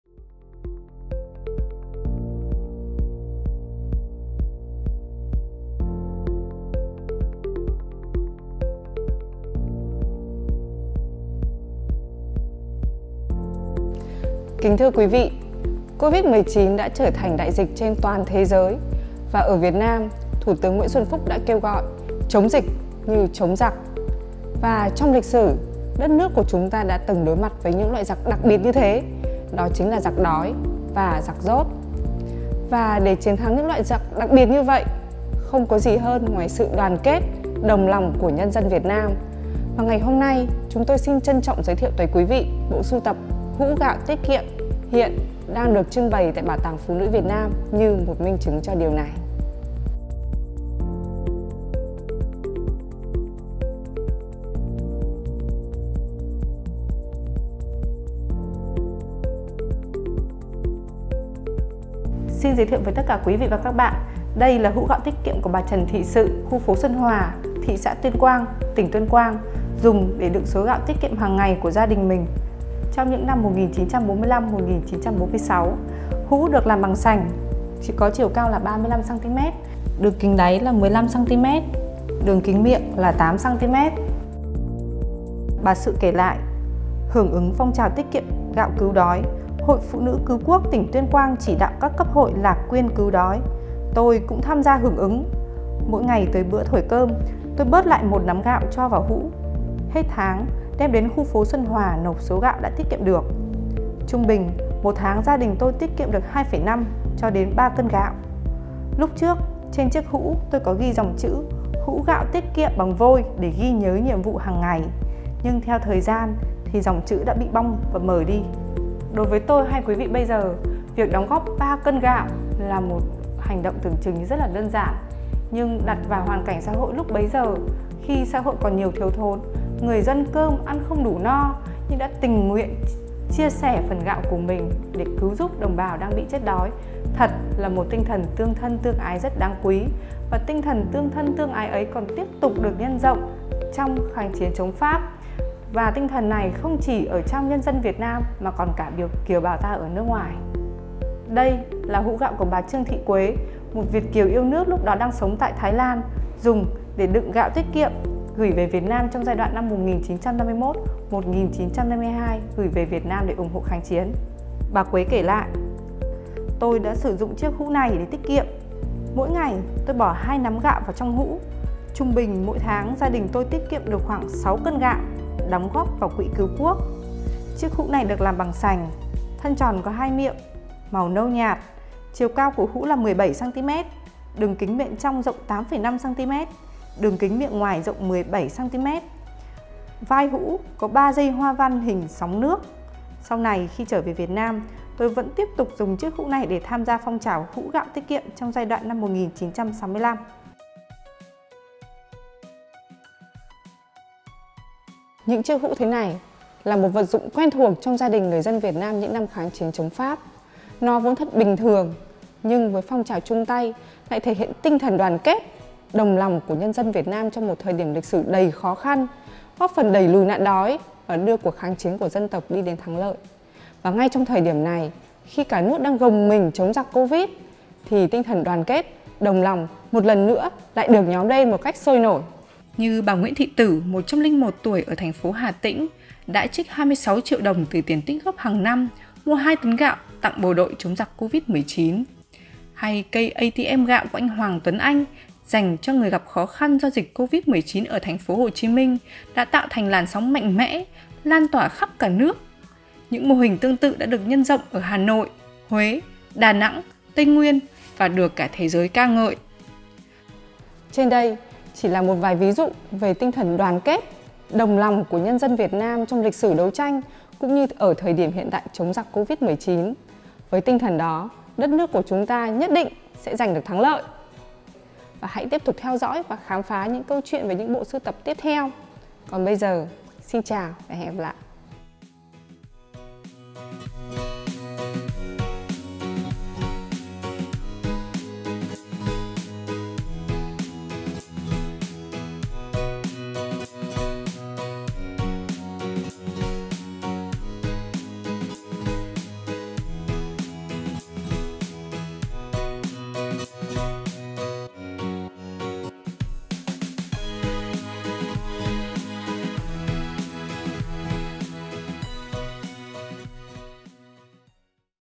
Hũ gạo tiết kiệm câu chuyện của tình đoàn kết - Rice jars The Stories of Solidarity
[English below]

Covid 19 đã trở thành đại dịch trên toàn thế giới, ở Việt Nam Thủ tưởng Nguyễn Xuân Phúc đã kêu gọi “Chống dịch như chống giặc”
Trong quá khứ, đất nước ta đã từng đối đầu với những loại giặc đặc biệt như vậy: giặc đói và giặc dốt. Và để đẩy lùi được loại giặc đó không gì khác ngoài sự đoàn kết đồng lòng của nhân dân Việt Nam.
Trong clip này, chúng tôi xin giới thiệu tới quý vị một bộ sưu tập “hũ gạo tiết kiệm” hiện đang được trưng bày tại Bảo tàng Phụ nữ Việt Nam như một minh chứng cho điều này.
Covid 19 has become a pandemic over the worldwide, in Vietnam, Prime Minister Nguyen Xuan Phuc has called being"Anti-epidemic is like fighting against the enemy".
In the past, Vietnam has faced such special types of enemies: the starvation enemy and the ignorance enemy.
In this clip, the Vietnamese Women's Museum would like to present our “Rice storage jar”  collection, which is displayed in permanent exhibition, as a testament to this solidarity.

Theo dõi Bảo tàng Phụ nữ Việt Nam/ Follow Vietnamese Women's Museum
🏢Địa chỉ: 36 Lý Thường Kiệt, Hàng Bài, Hoàn Kiếm, Hà Nội